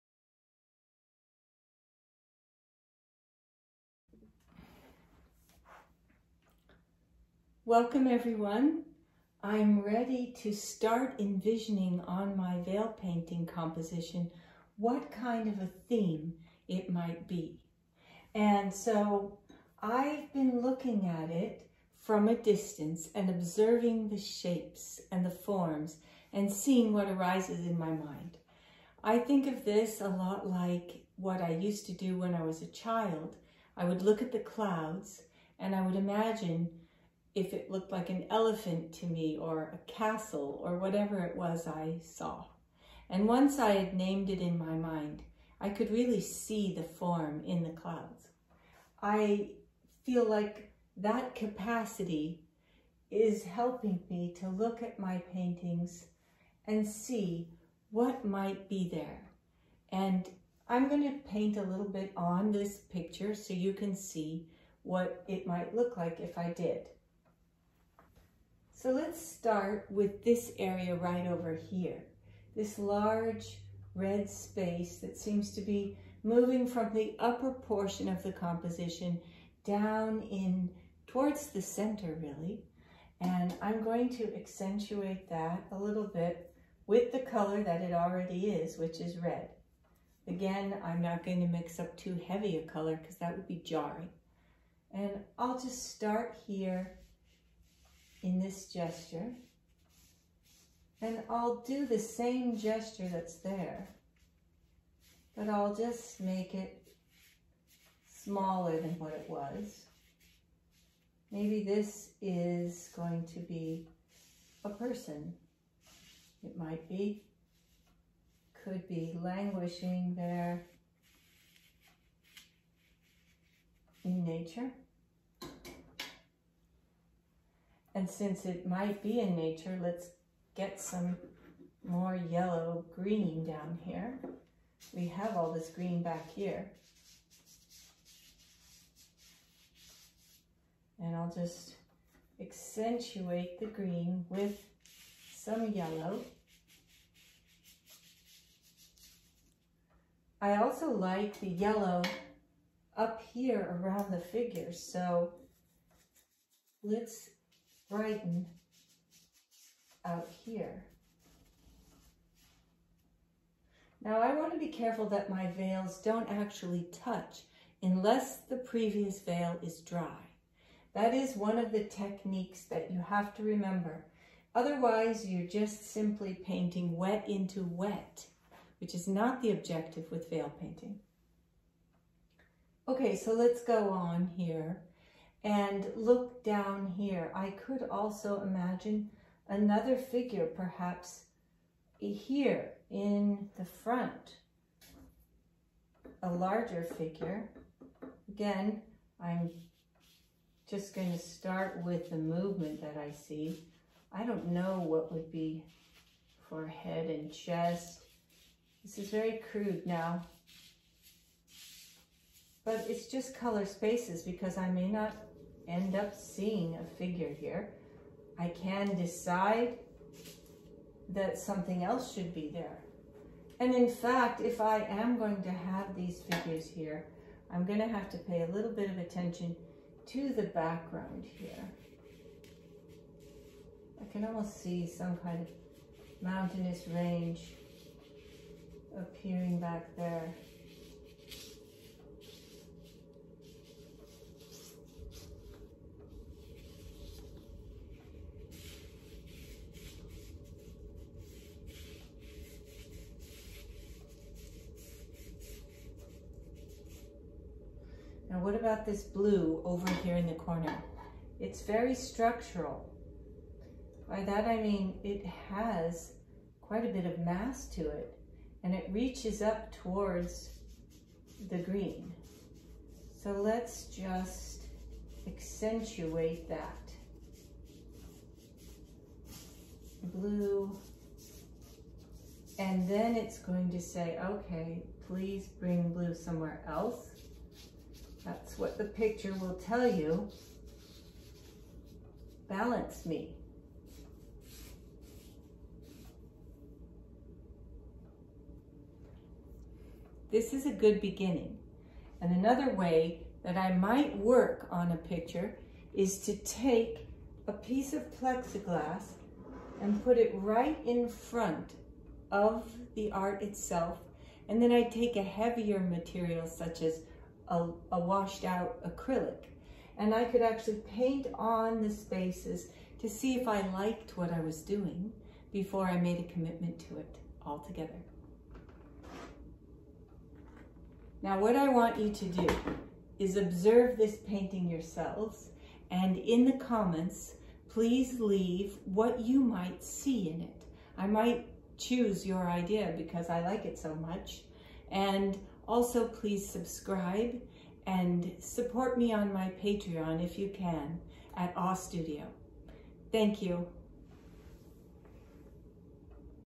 HOW TO IMPROVE YOUR IMAGINATION WITH VEIL PAINTING
This Veil Painting Demonstration will teach you how to balance an abstract color composition and learn how to develop your imagination of what the painting theme may become. Janet Lia invites you to participate in finding what the painting is by asking you to tell her what you see in the colored spaces and tonal forms.